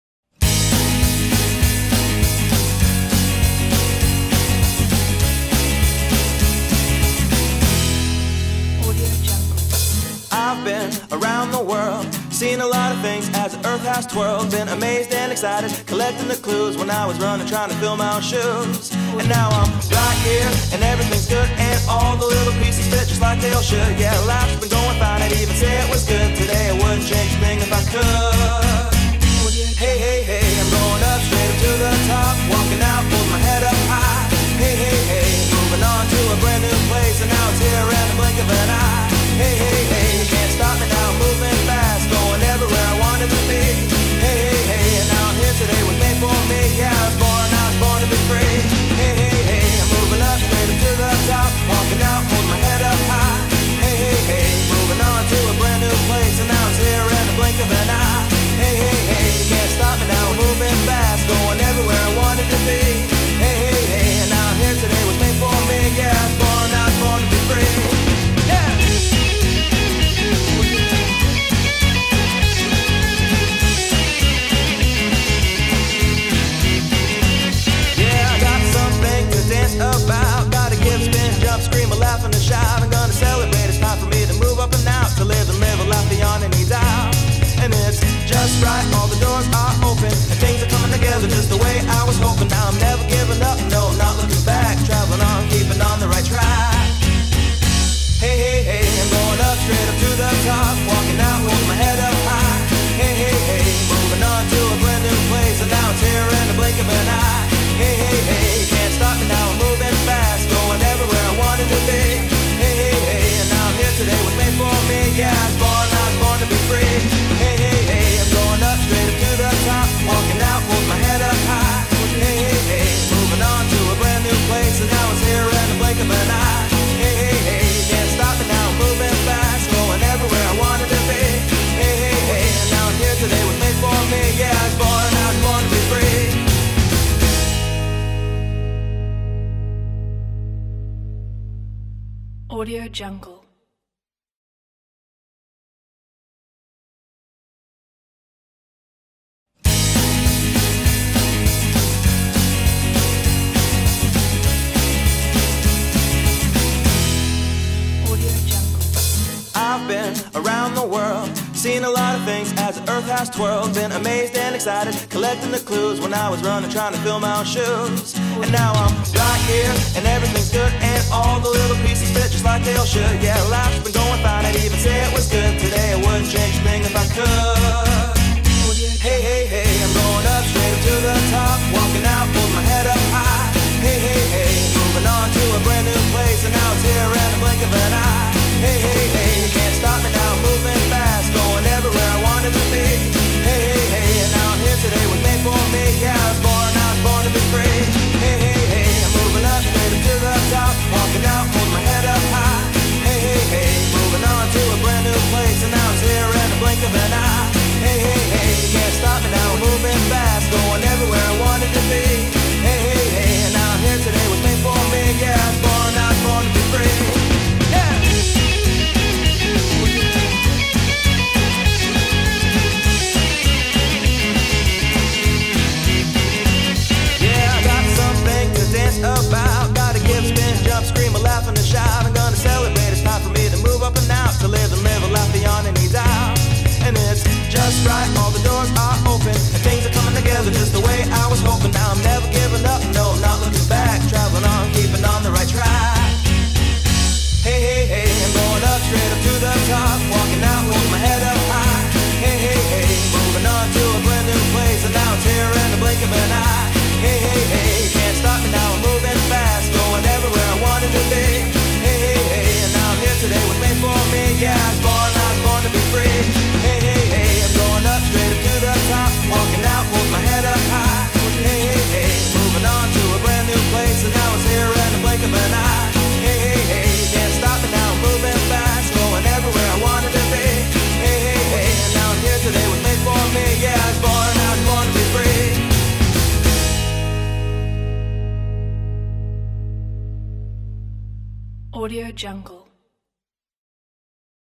Lovely Slides II-After Effects Template Videohive
Main Features

    Beautiful, vintage design
    Animated intro & outro
    “Film burns” and color corection settings are also included
    4 unique Insignia designs
    100+ placeholders
    Modular structure
    No plugins needed
    Detailed 25+ min video tutorial